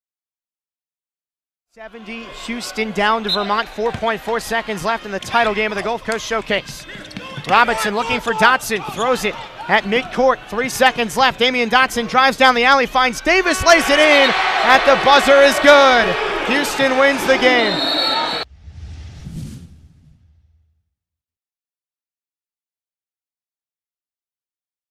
Houston Cougars Claim Gulf Coast Showcase Title Against Vermont in Final Second
Junior forward Devin Davis banked in a layup as time expired to lift the University of Houston Men's Basketball program to a 72-71 win over Vermont in the Gulf Coast Showcase championship game on Wednesday evening inside Germain Arena.